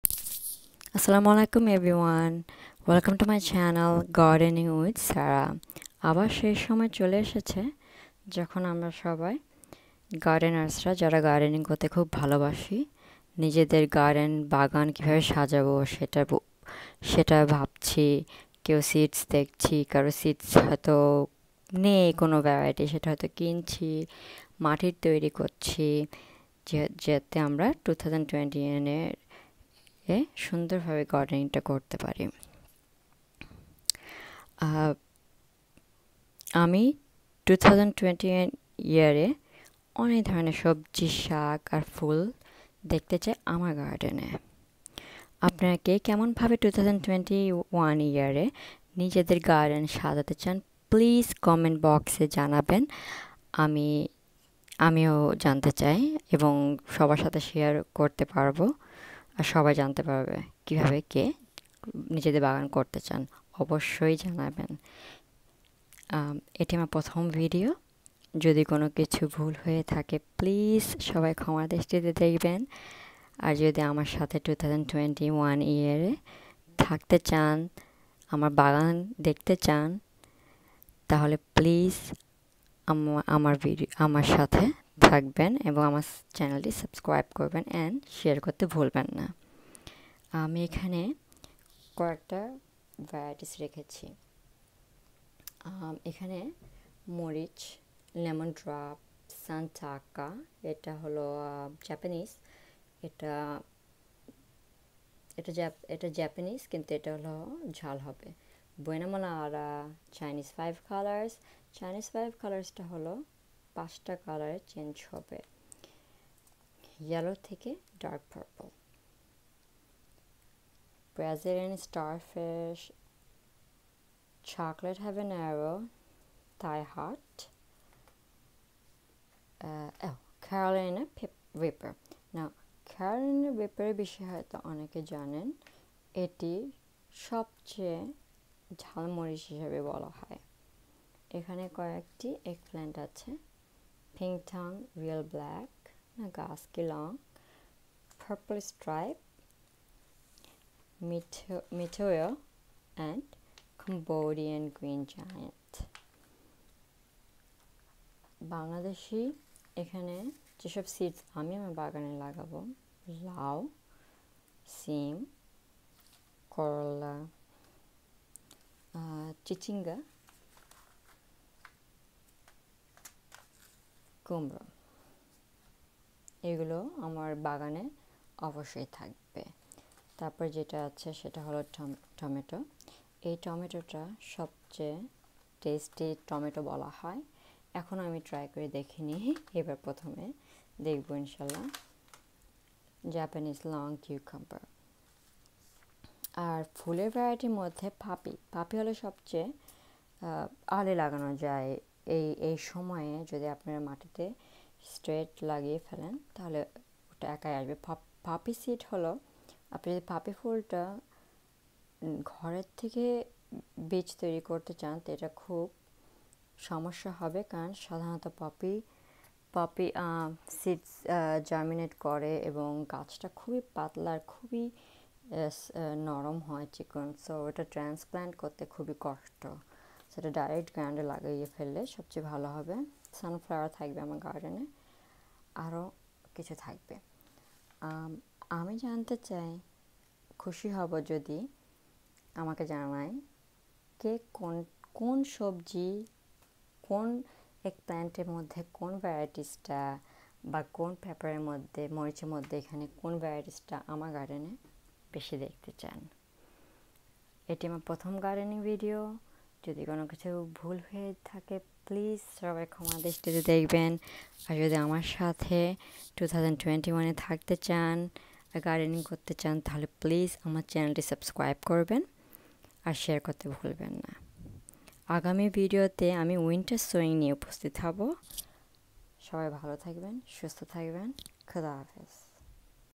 Welcome to Gardening with Zahra 2021 garden seeds - 2021 e jei sobji o ful amar bagane thakbe
Hello Everyone! Welcome to my new channel called Gardening with Zahra. I have discussed some seeds I am growing this year. I like to grow organic vegetables in my backyard. Most of my seeds are from Baker Creek and Botanical Interests. I
mostly use heirloom seeds. I look forward to
growing lots of varieties of peppers this year. Hope I could bring some basic knowledge for everyone who loves gardening and for new gardeners as well.

Pepper: Lemon Drop Pepper, Santaka, Buena Mulata, Chinese Five Color, Carolina Reaper, Brazillian Starfish

Eggplant: Edrine Purple Striped, Cambodian Green Giant, Mitoyo, Nagasaki Long, Ping Tung,
Real Black

Bangladeshi:Sim, Bittermelon/korolla, Pumpkin/misti kumra, Gourd/lau, Snake Gourd/cichinga, Luffa, Ridge Gourd/jhinga, Malabar Spinach/pui sak, palon sak/Spinach, borboti/yard long bean / noodle bean
Other varieties:Okra, Cucumber, Tomato, melon
Flowers: Sunflower, Poppy, Zinnia